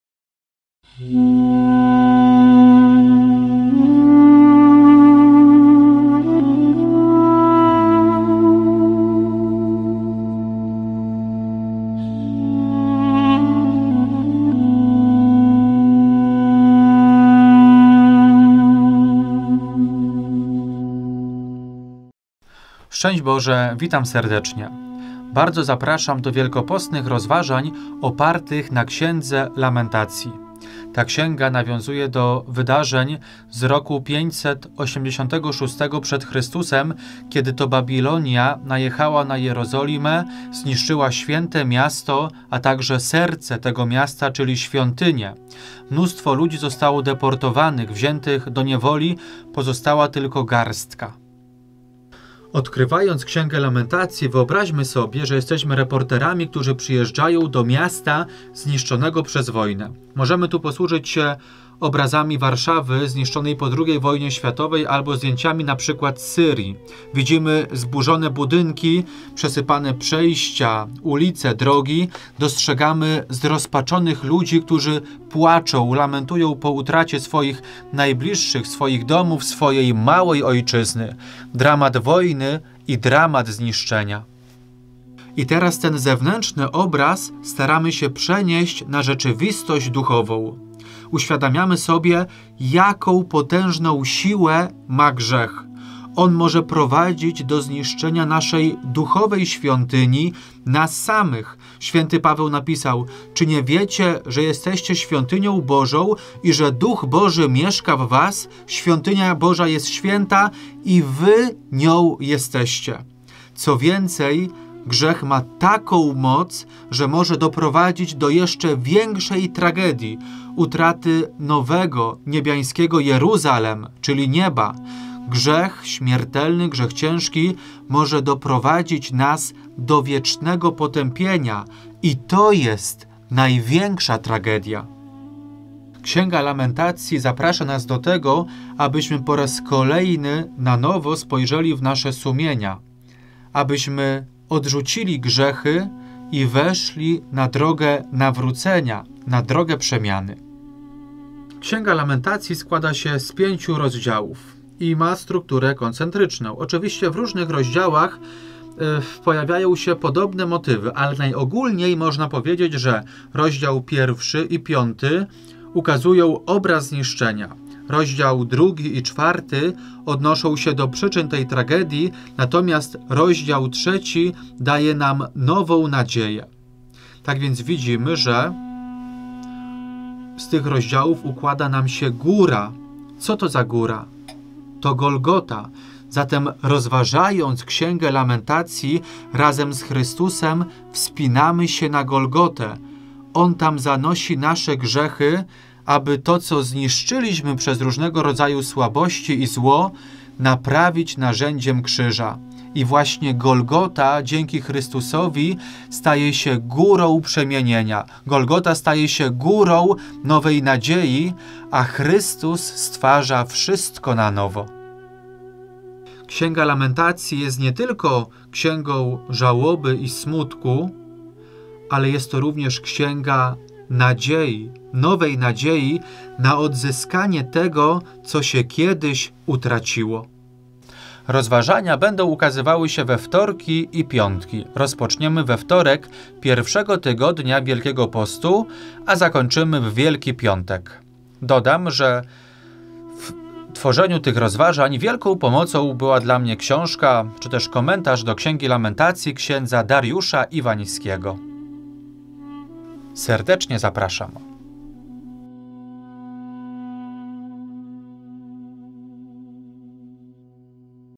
PROJEKT LAMENTACJE [#1] - ZAPOWIEDŹ
Zapraszam do wielkopostnych rozważań  opartych na Księdze Lamentacji połączonych z błogosławieństwem.